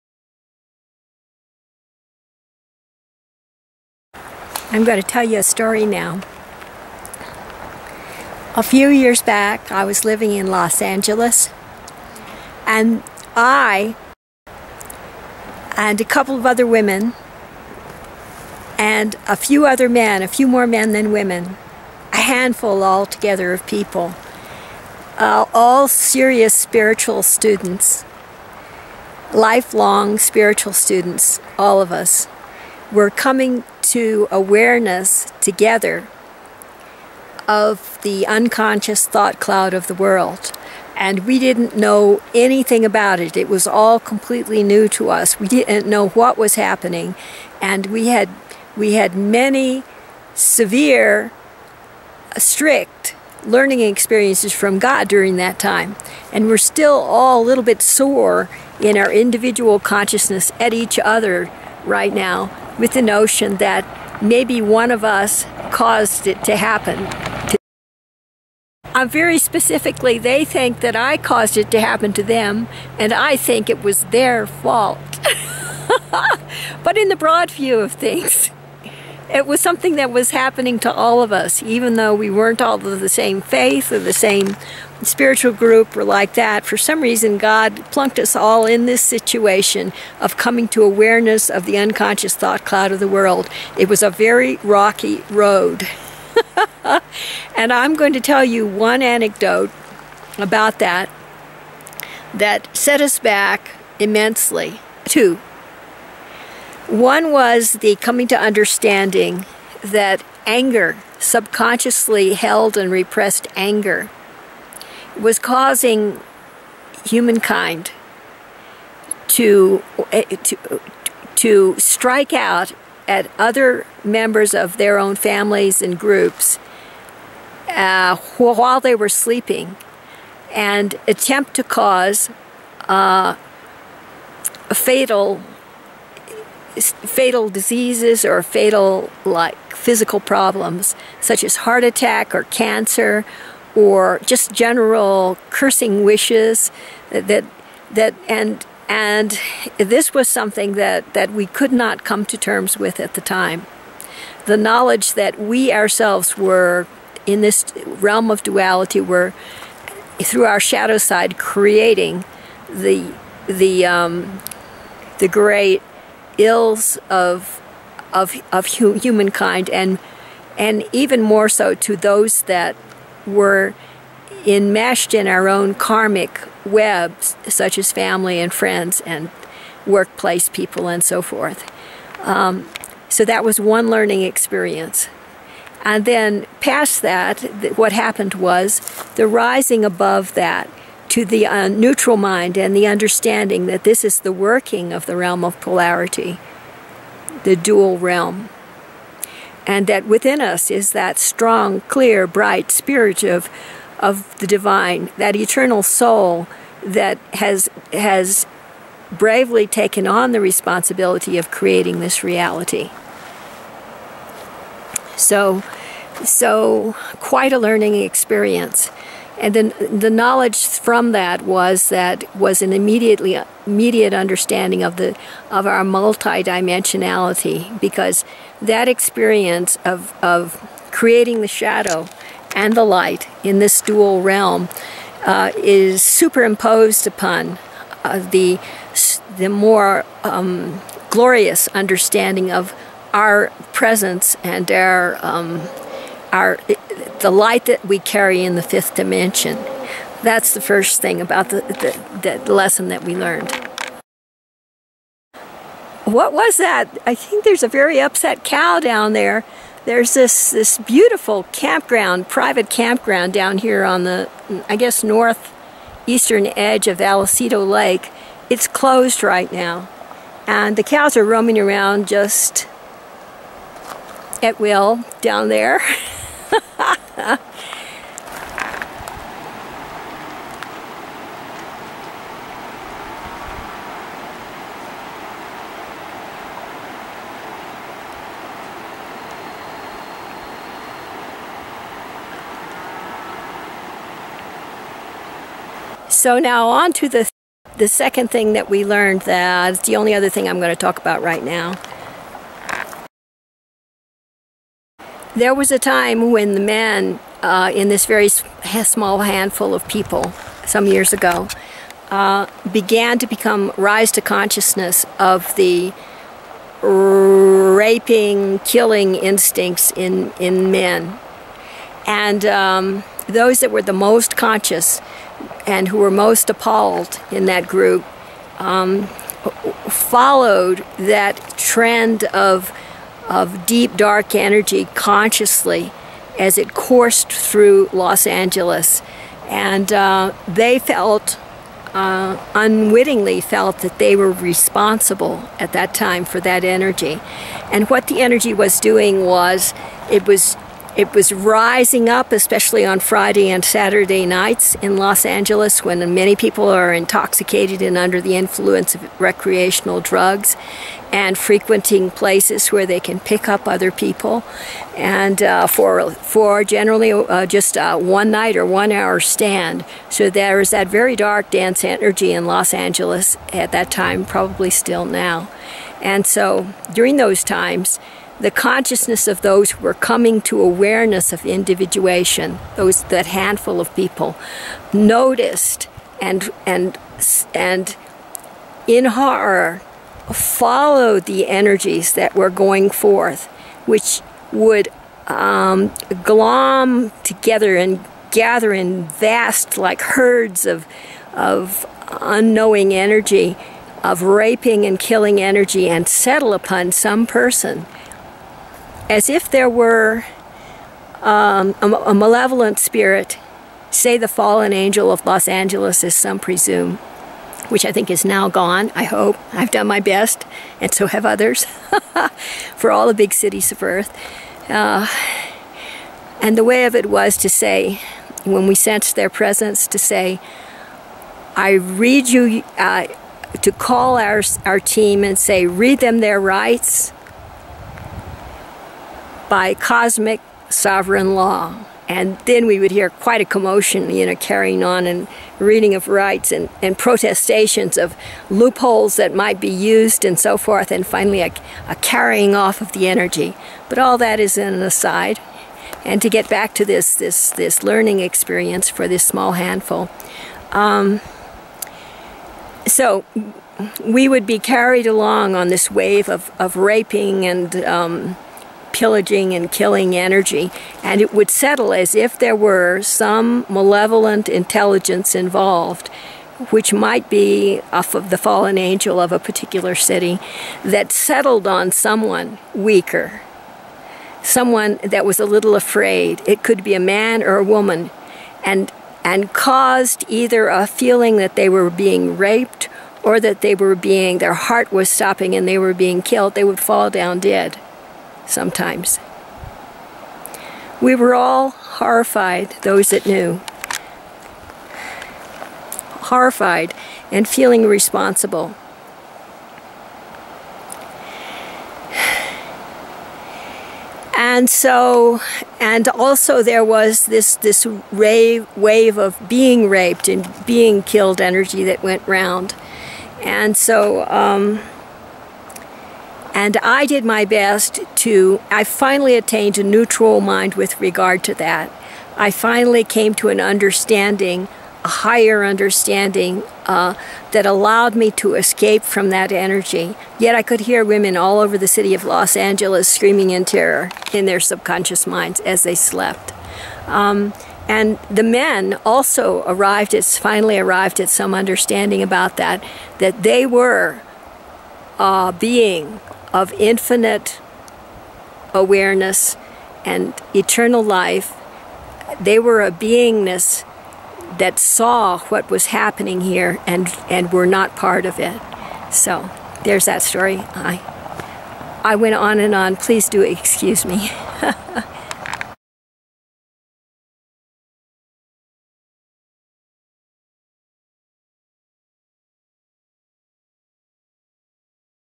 Personal Recollections: Rising to Awareness in LA ... by Alice B. Clagett
Dear Ones,
Filmed on 19 September 2015; posted on 21 September 2015; revised May 2016 and transcribed 12 October 2018

Full title: Personal Recollections: Rising to Awareness in the City of the Angels

Citation for the video: Read them their rights, by Cosmic Sovereign Law!